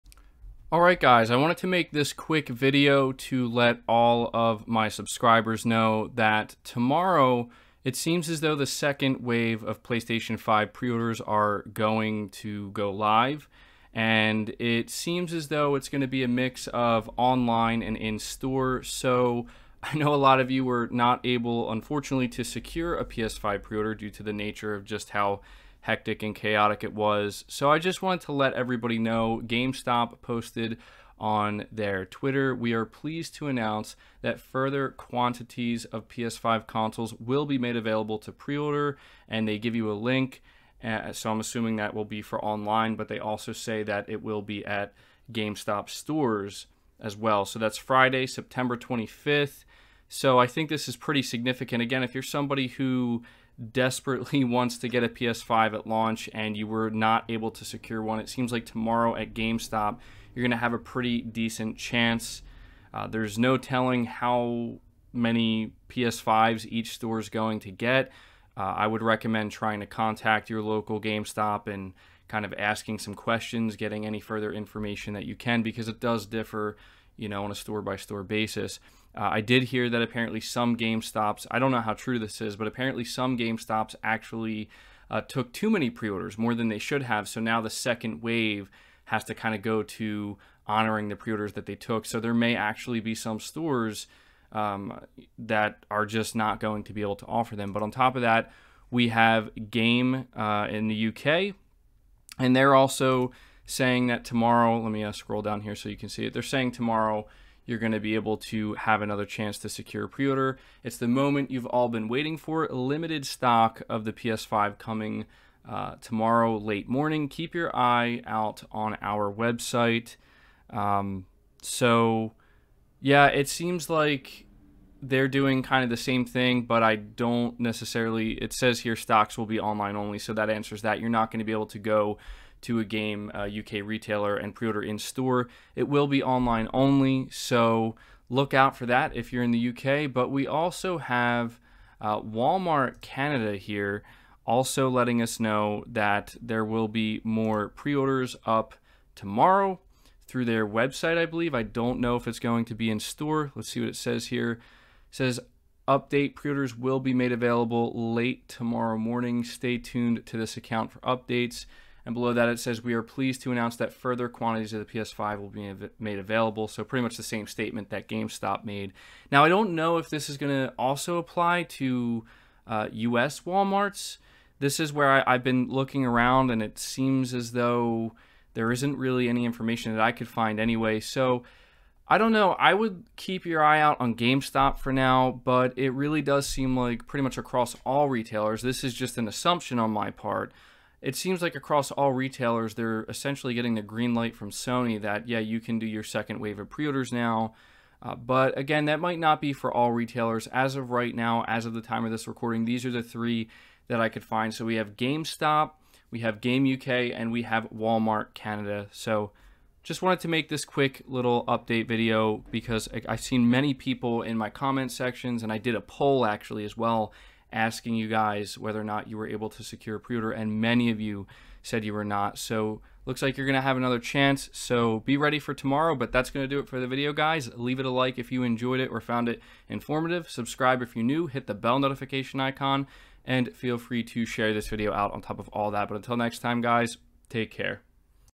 Second Wave Of PS5 Pre-Orders Are Going Live Tomorrow | PS5 News Update
Today Sony and multiple retailers announced that the second wave of PS5 pre orders will be going live tomorrow. It will be at retailers such as Gamestop, Game UK , and Wal Mart and stock will be very limited it seems. It will also be in store and online. A lot of PlayStation fans who want a PlayStation 5 at launch were not able to secure a pre-order so tomorrow will be your chance.